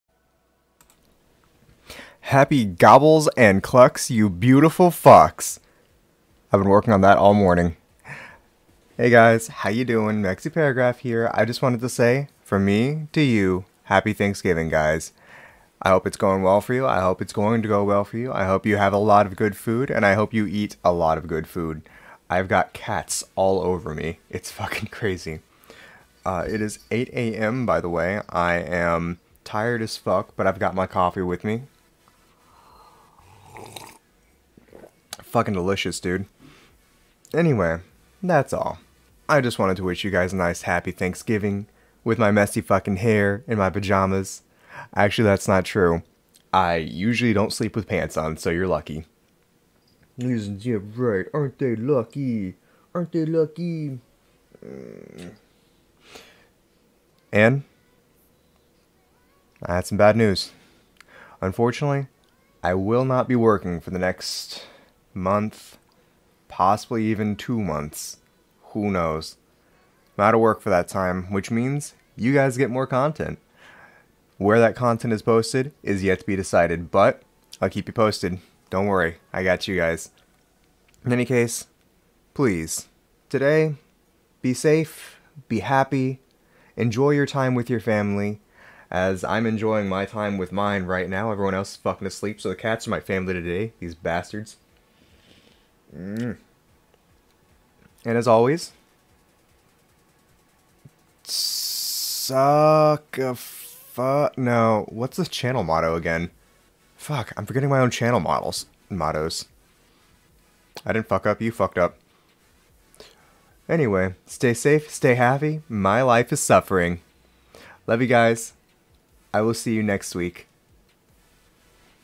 Happy Thanksgiving!
From me to you, have a happy Thanksgiving, and remember to spend lots of time with the family of your choosing. Also eating. Spend a good amount of time eating.

Like on a scale from 1-10 about a 4, kind homey, salt of the earth kind of content. I know I'm at least hitting like a 3.5